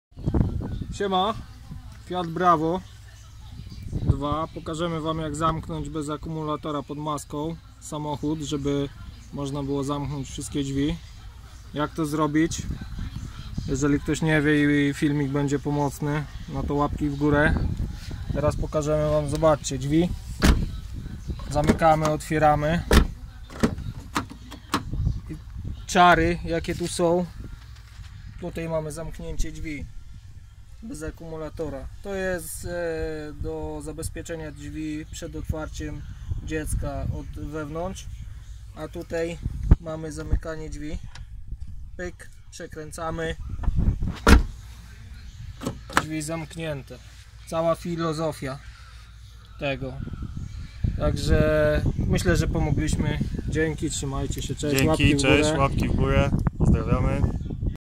Fiat Bravo 2 ! Padnięty akumulator ! Jak zasunąć auto .
Witam , każde drzwi zamykamy tym sposobem oddzielnie.  Drzwi od kierowcy zamykamy kluczykiem. Żeby otworzyć otwieramy drzwi kierowcy kluczyki a resztę drzwi od środka za klamki . Lub po włożeniu akumulatora centralnym zamkiem . Pozdrowienia .    I close the car without the key from the inside.